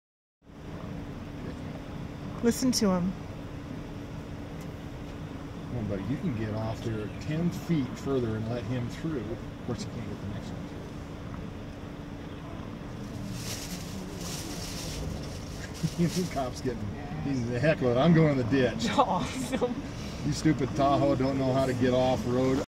Yellowstone 2015 buffalo swim
August 2015.  Family vacation to Yellowstone.  Watching buffalo swim across the river in Hayden Valley